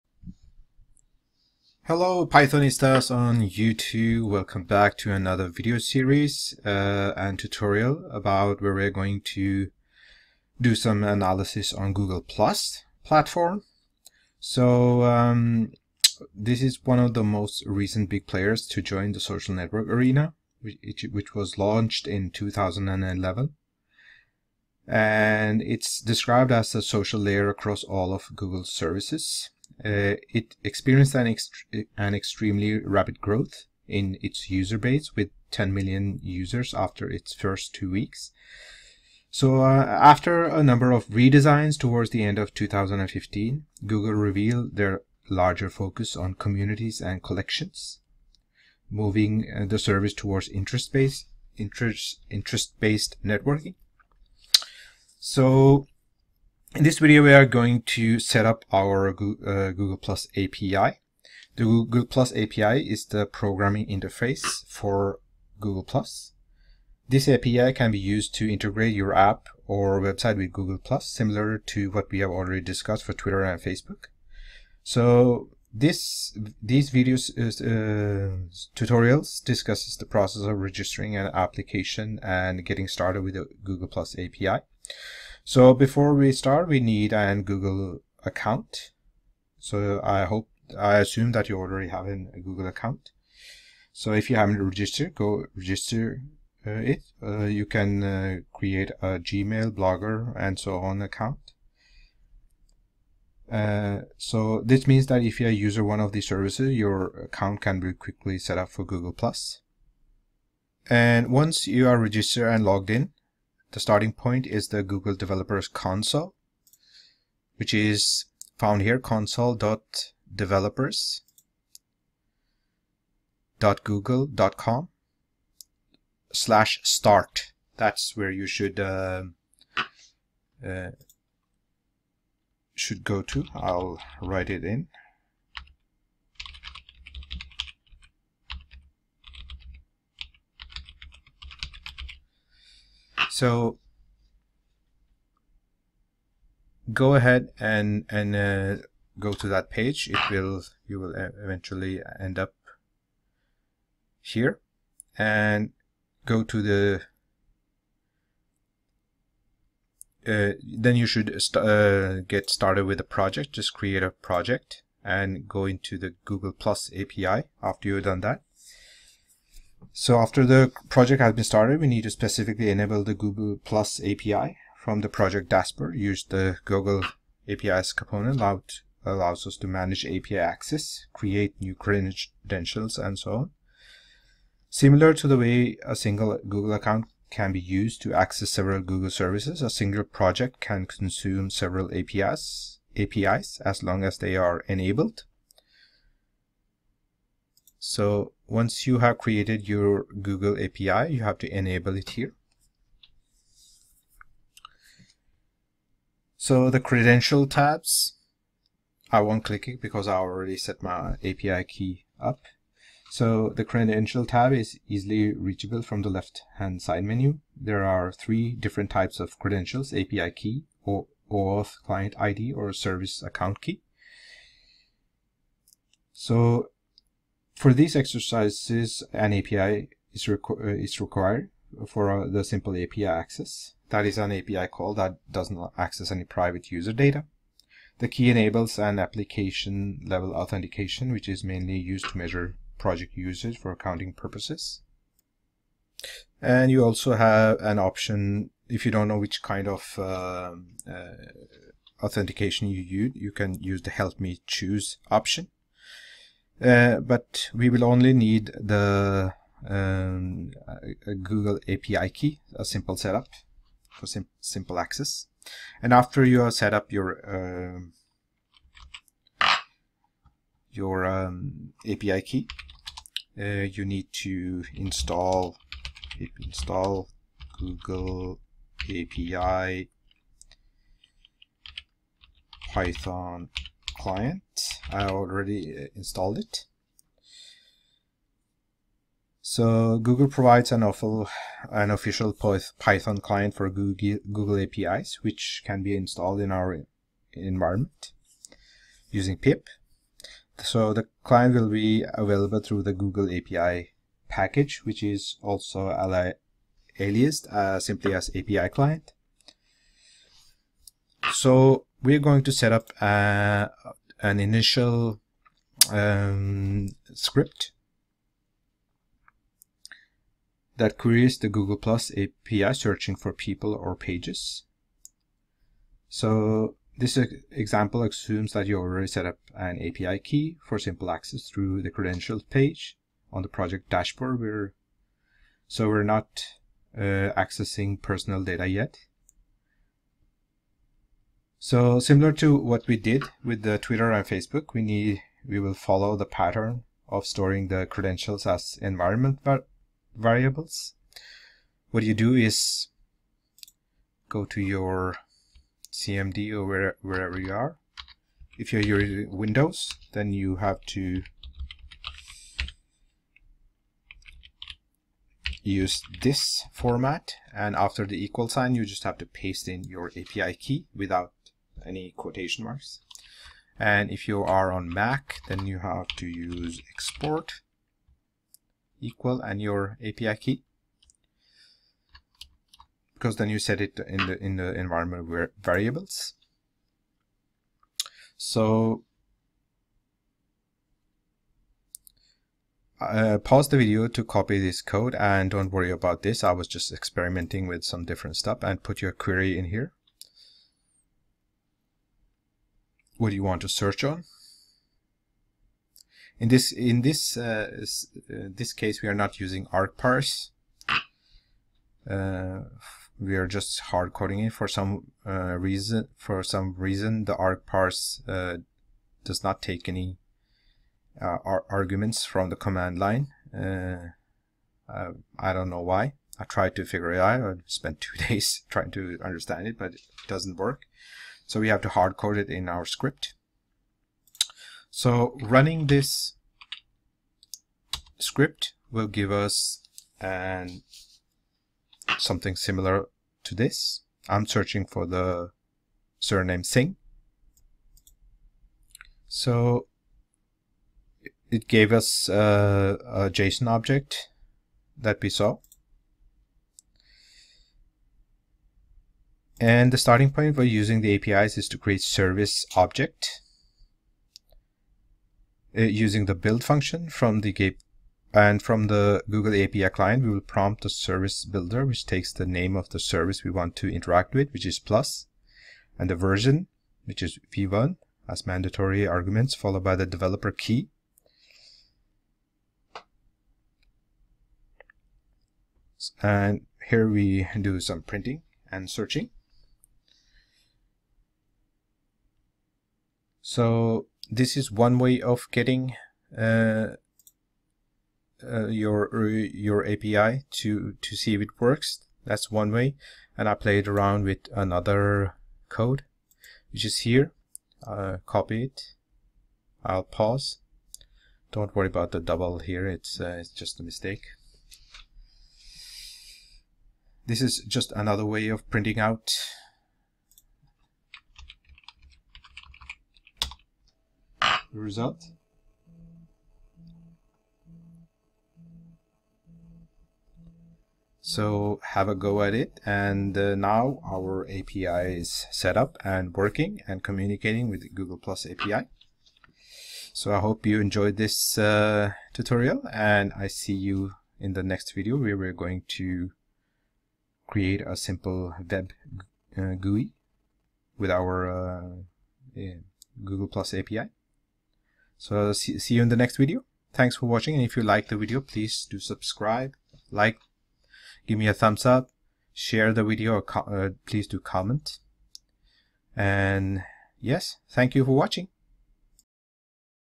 Mining data on Google+ with Python: 1- Setting up Google+ API with Python
We will set up our Google+ APi with creating an API key and putting it to test to do some initial testing to see if it works.